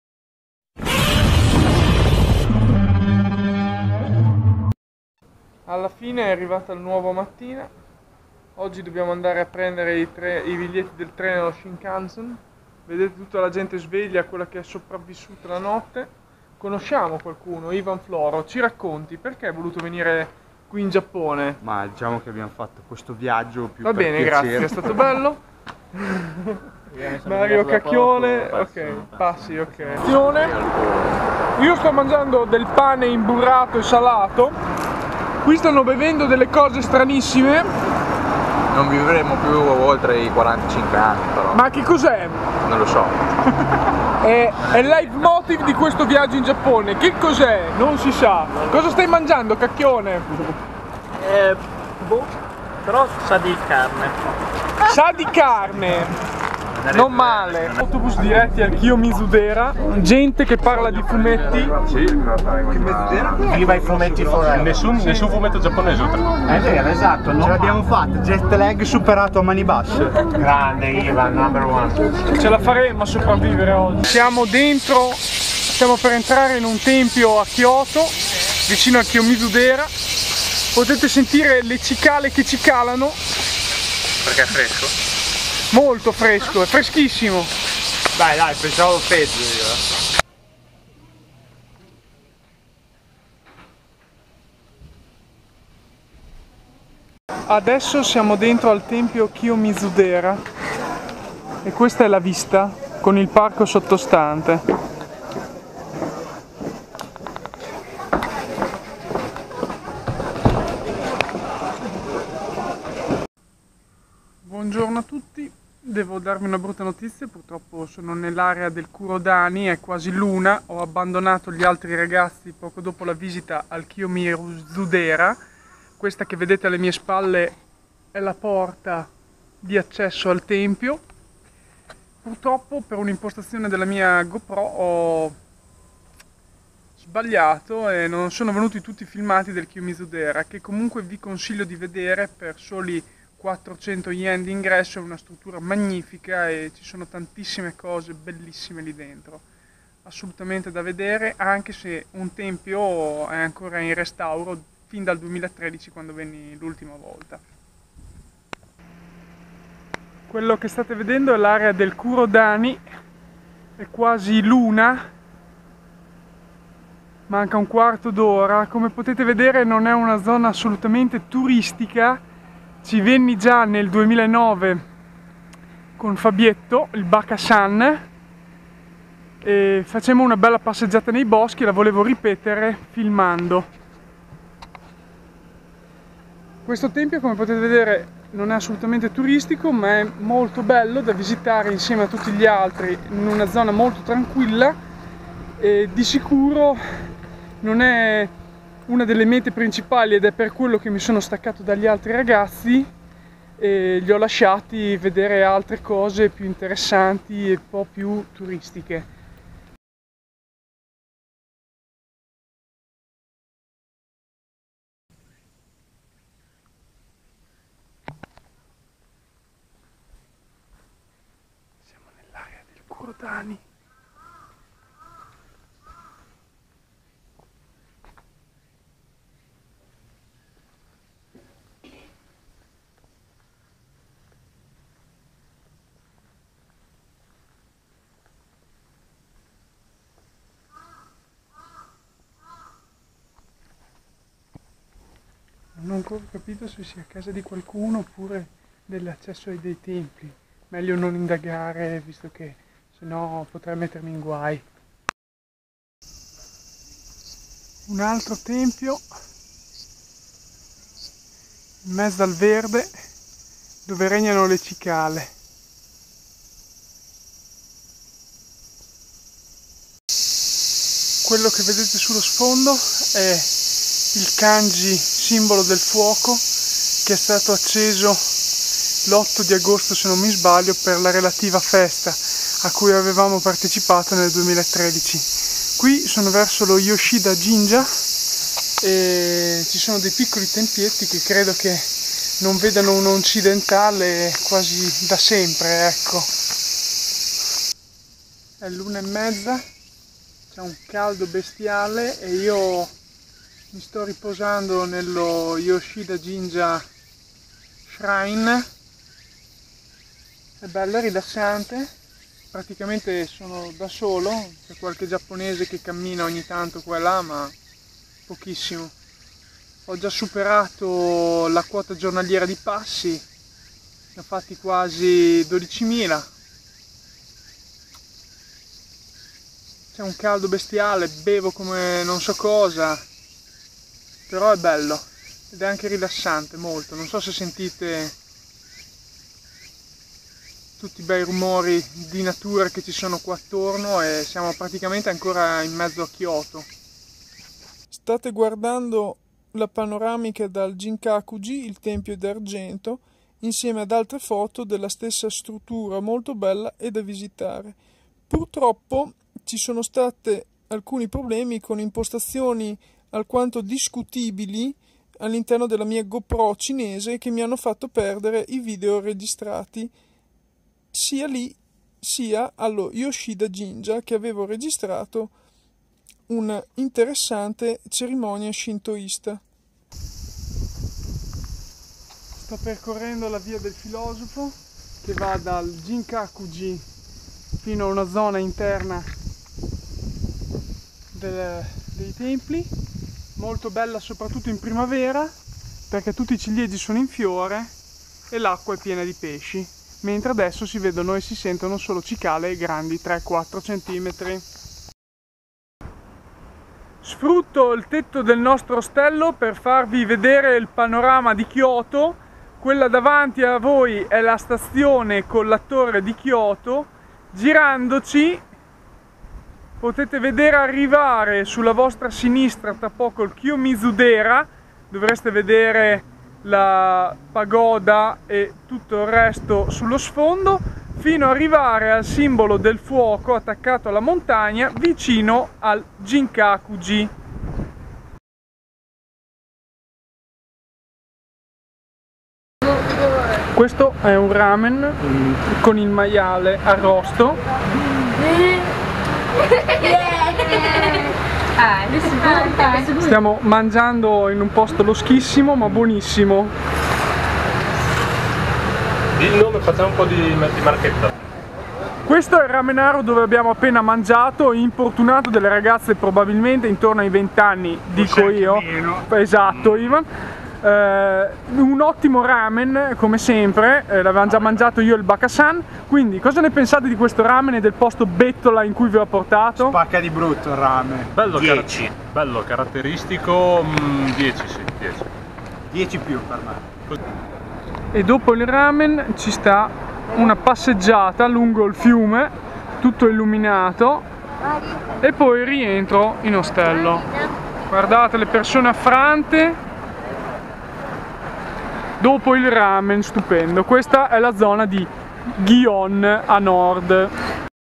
Giappone 2016 - Day 2 [MTE]
Un passaggio a Hiragashihama, nel nulla tra Kurodani e Yoshida per finire al Ginkakuji.
Con una ottima cena di ramen dal "losco".
Un giro che vale 15 km all'incirca.
Sempre bella Kyoto anche se estremamente calda.
State connessi per il giorno 3.